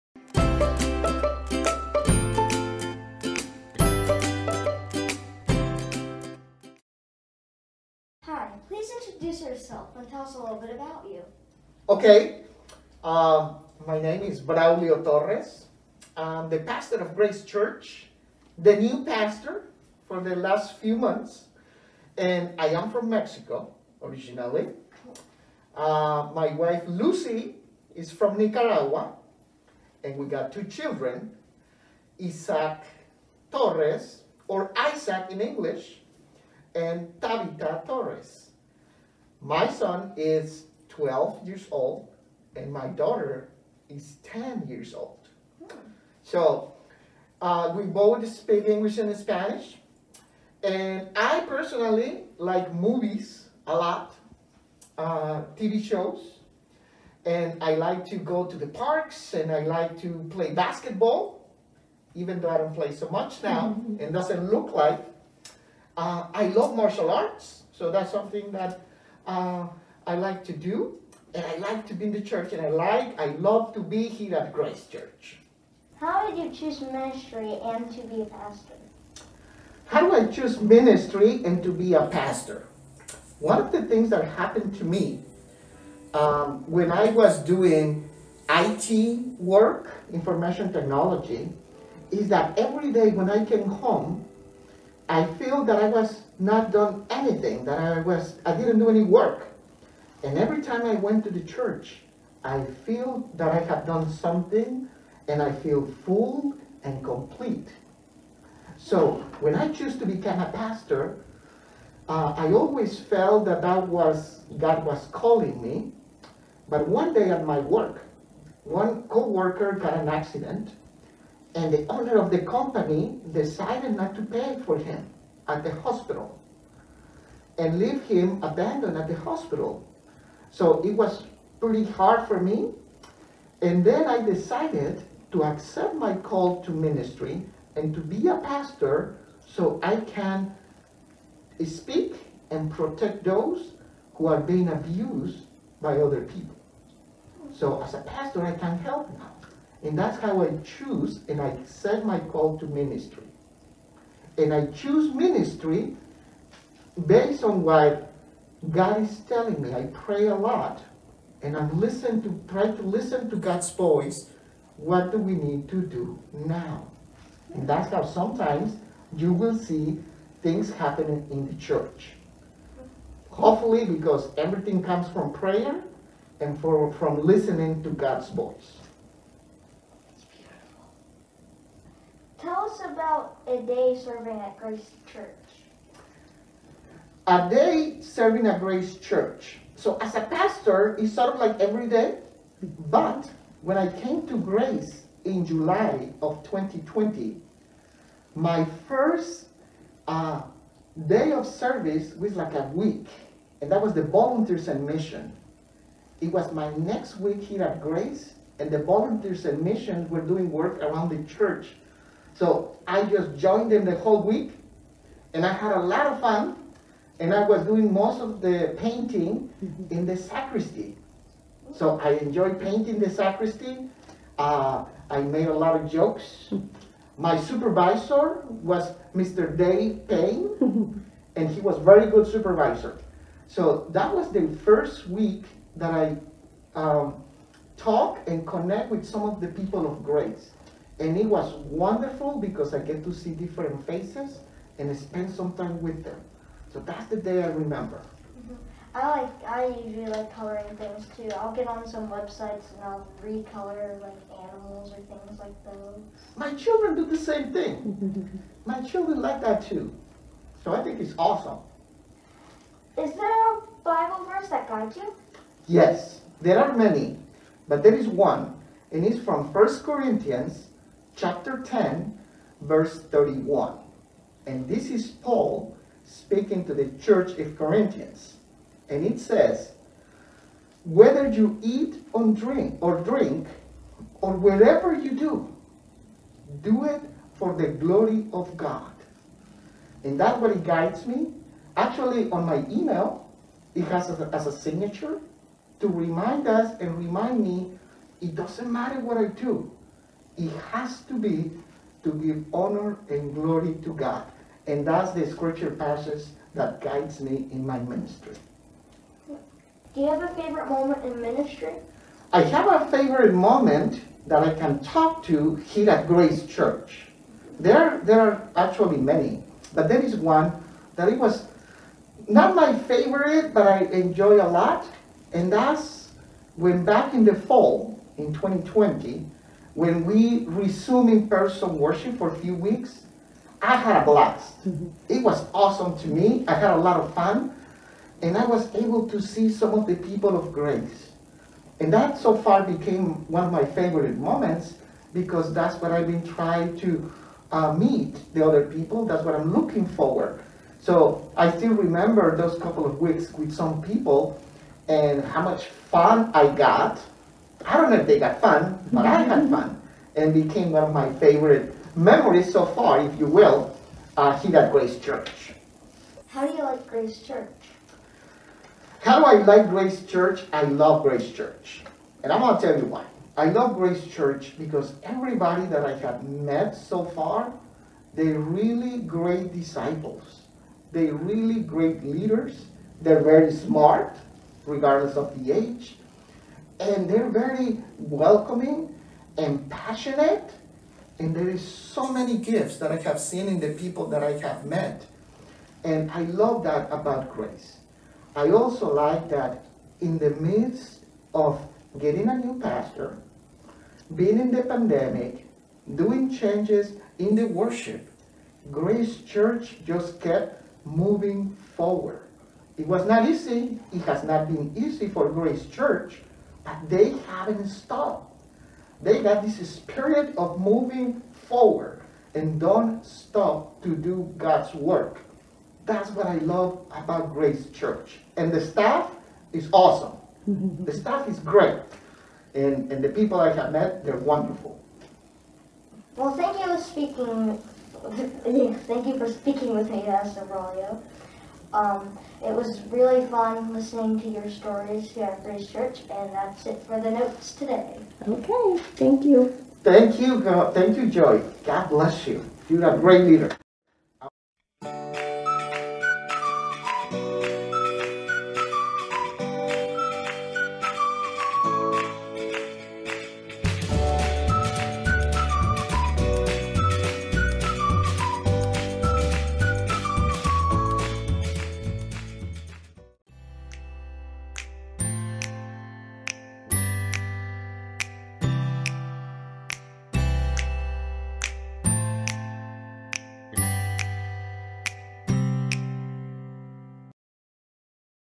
Family Moment Ministry 6-6-21
This week in Grace UMC's Family Moment Ministry, Joey sits down for a chat with Pastor Braulio about his ministry at Grace, his family, what inspires him, and hobbies he has. Join in to learn more about our Pastor!